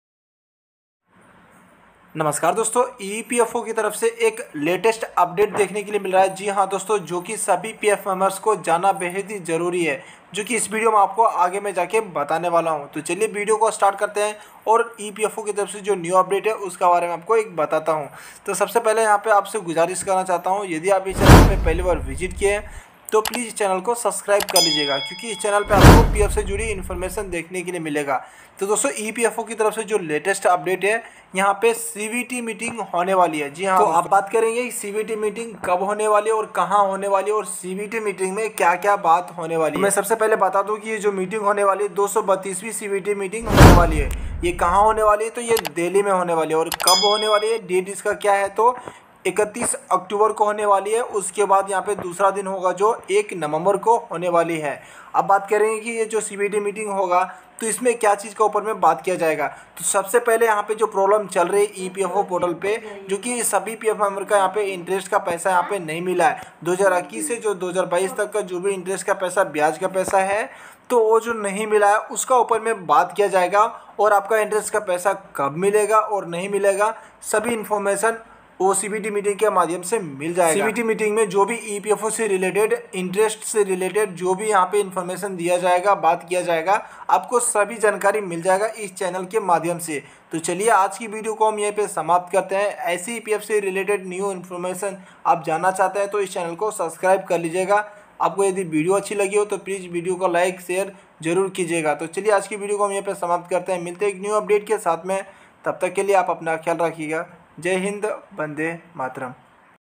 EPF Latest Update All EPS Members सभी पीएफ मेंबर के लिए जल्दी वीडियो देखेंtechnical_mp
doston Aaj hi video mein baat karne wala EPFO ki latest update ke bare mein

yadi aapka bhi interest ka paisa Nahin a raha hai to

232 CBT meeting Mein Sab kuchh clear ho jaega

C.B.T Meeting Updates .👇👇👇

☄️232 C.B.T Meeting hone Wali Hai

☄️ Location.. Dehli Me Hogi C.B.T Meeting

☄️Date: 31 Oct Or 01 Novem Ko Hone Wali Hai

☄️EPF PF EPS EPFO Se Juri Information

☄️ PF Interest amount credited Kab hoga.??

यह सभी बातों के अलावा और सारे बातों पर चर्चा किए जाएंगे।।।

धन्यवाद 🙏🙏

epf_pf_interest__pf_passbook